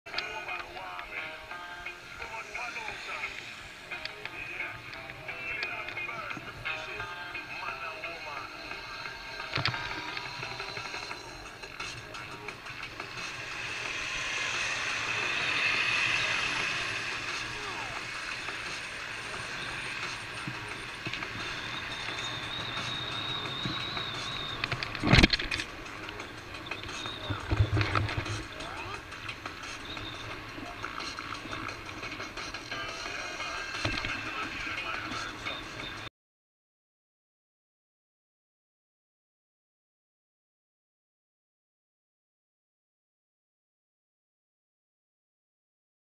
illegal pass + unsignaled pass
Westbound on Lake Street just west of 10th Street, Minnesota license 481 NEL passed illegally closely without signaling.